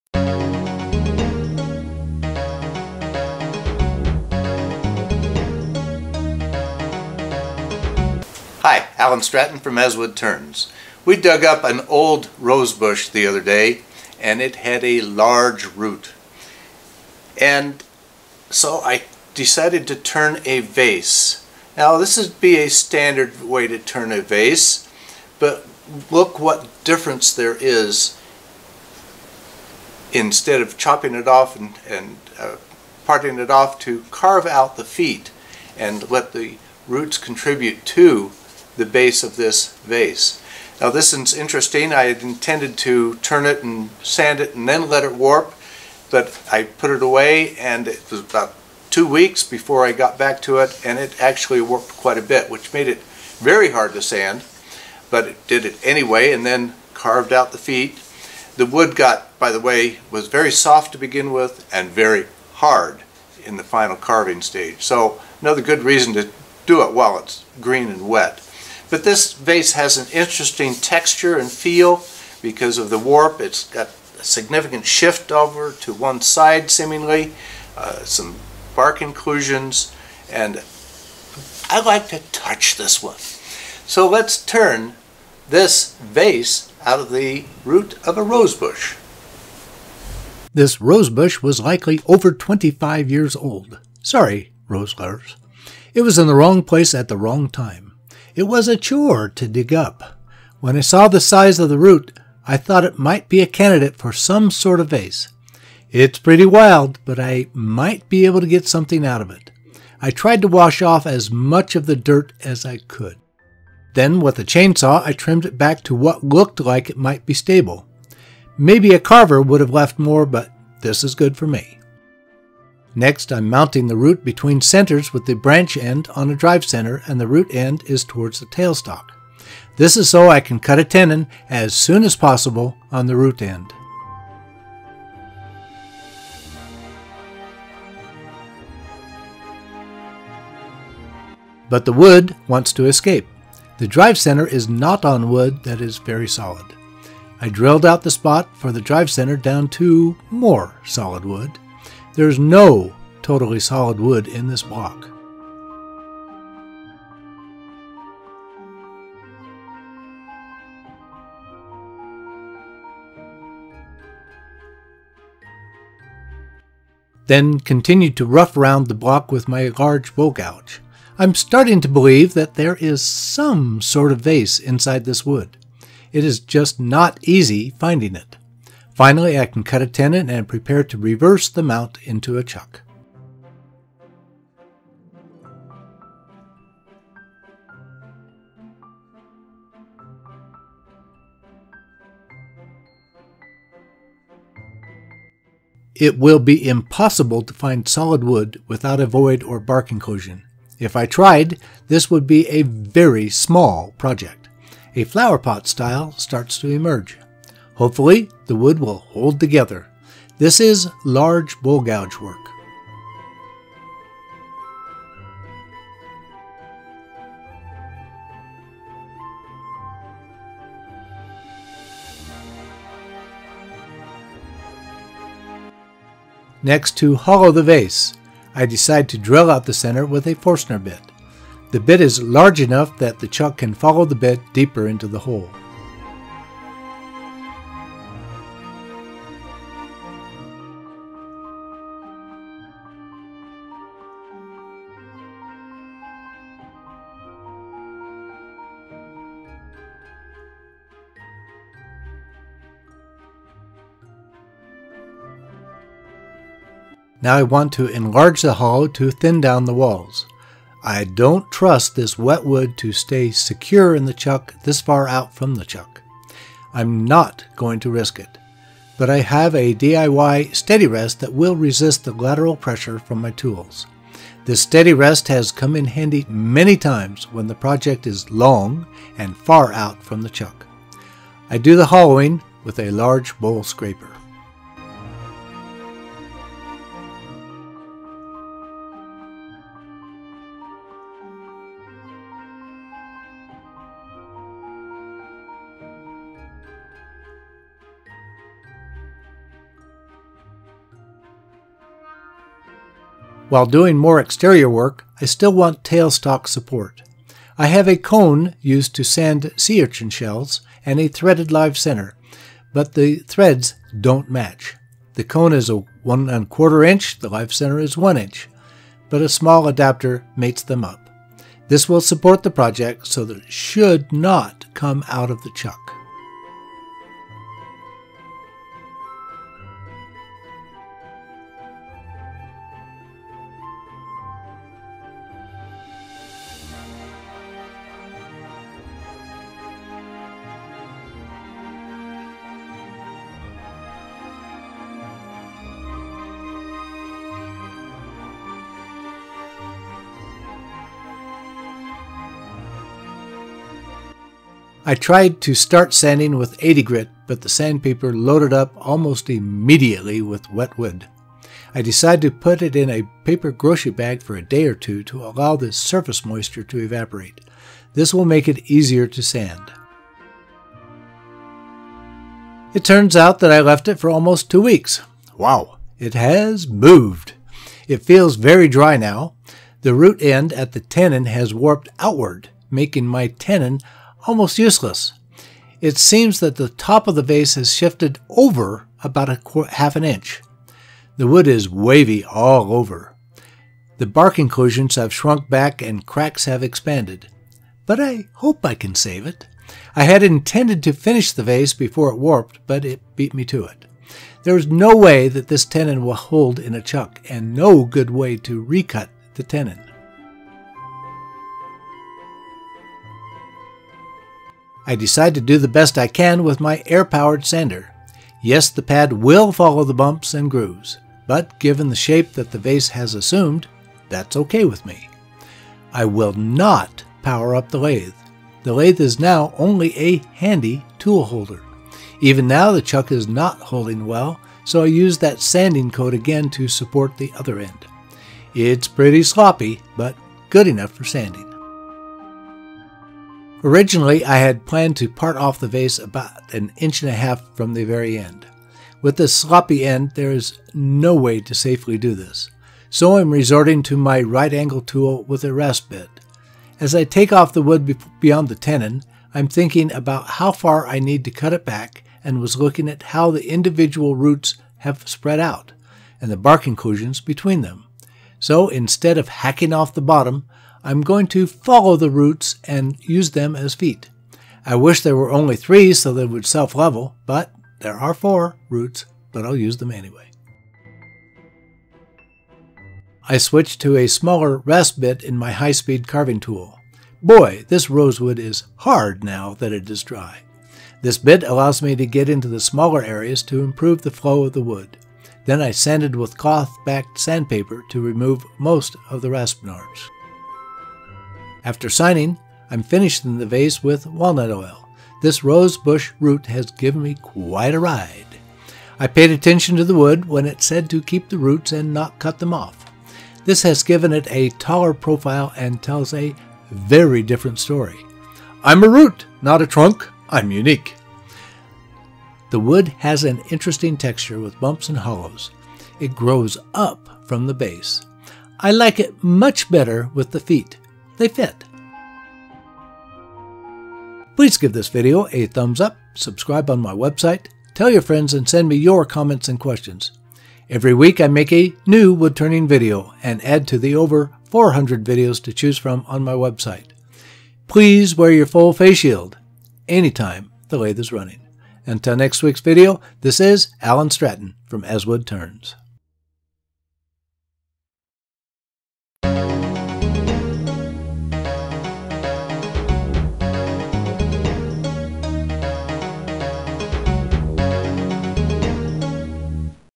Woodturning A Rose By Any Other Name
While redoing some landscape in my yard, I removed an old rose bush including the root ball. This root was a bugger to dig out. When I realized how big it was, I just had to try turning it.

I've turned other smaller rose roots previously. They are always unique experiences - sometimes frustrating experiences. I planned to turn this one completely while wet. However, when I started sanding it, the sandpaper loaded up with the wet wood almost immediately. I put it away in a paper bag for a day until the surface moisture could evaporate. Then I could sand it and let it warp.

My day turned into two weeks and it beat me to the warp stage. This caused other problems in trying to sand and finish this vase.

However, the delay also generated an opportunity to change the design. The individual roots at the bottom had spread. Incorporating these roots into the design instead of parting them off radically changed (and improved) the result.

I'll keep a lookout for another large rose root.

My vase is 7 inches tall and about 4 inches in diameter, finished with walnut oil. It has developed its own character.